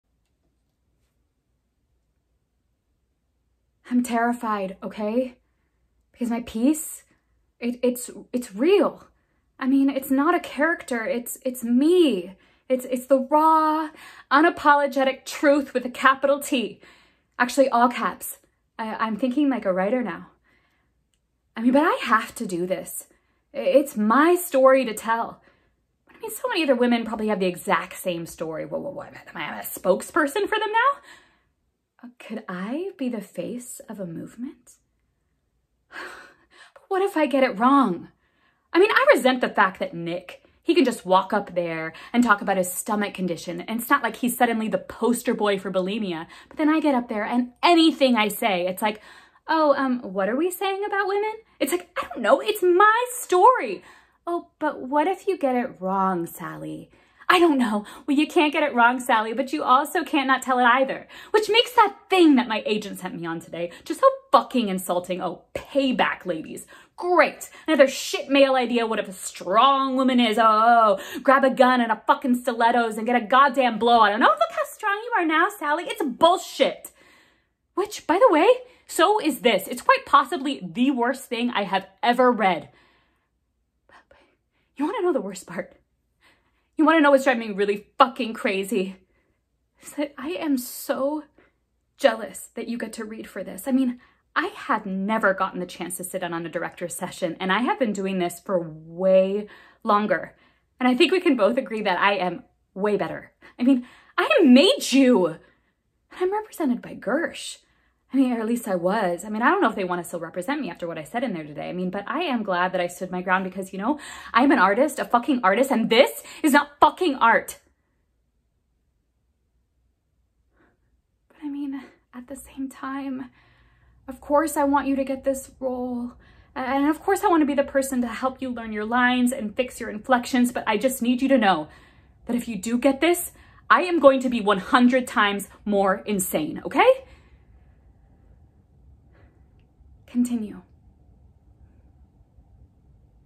Monologue from Barry (Sally!)
As a part of my self-tape May challenge, I'll be posting some of my monologues here! Here is one of my favorite monologues from the amazing show Barry. Hope you enjoy!